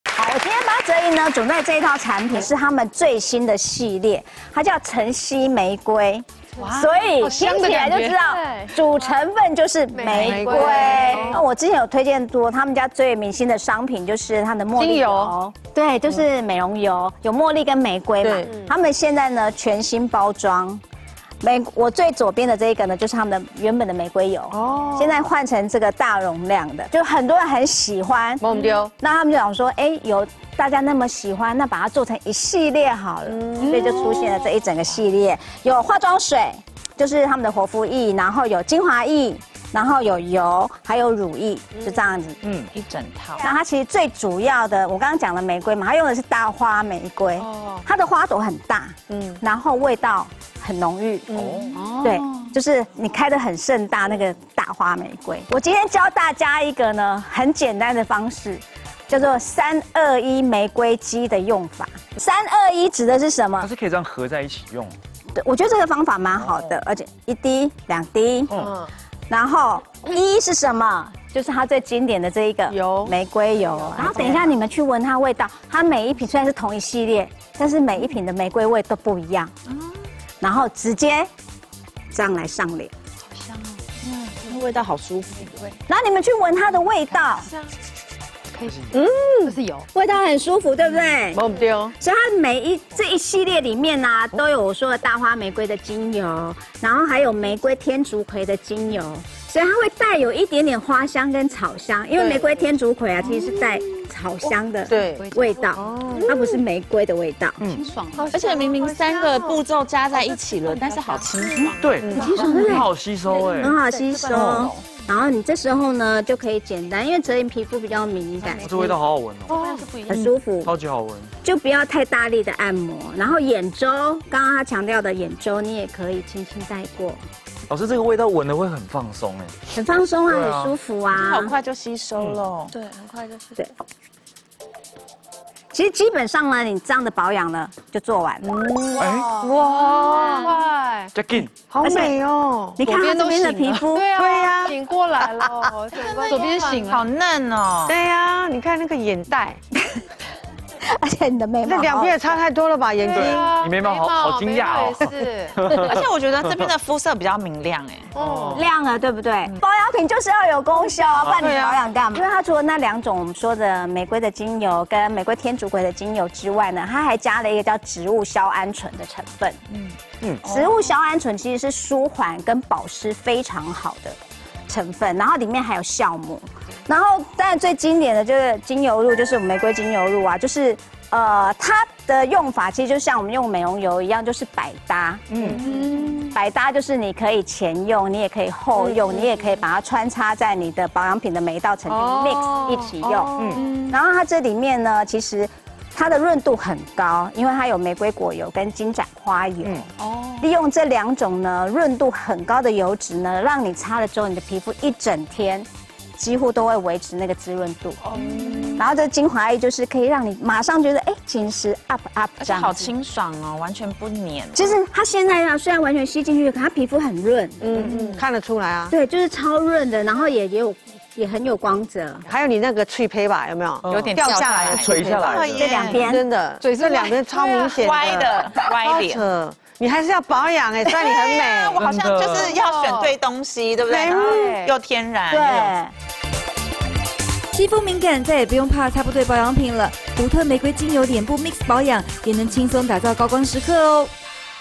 頂級玫瑰保養新選擇 Blueseeds晨曦玫瑰煥顏系列
一瓶玫瑰就已經夠奢了
更何況四瓶 OMG🤩
於清晨採摘盛開180度的大花玫瑰🌹
香氣馥郁內斂 有如漫步在玫瑰莊園
@blueseeds 晨曦玫瑰換顏 系列產品
更推出 321晶透玫瑰肌保養法
💧早晨: 活膚液3滴+精華液2滴+精油露1滴
💧夜晚: 活膚液3滴+乳液2滴+精油露1滴
讓你的肌膚早晚都水嫩與光澤~~

藍心湄、速玲 「年齡、豐富了我的人生」！

『唐綺陽 開運系列』有看有準 有看有加持~！
『髮神吳依霖』拯救亂髮，看這個就知道！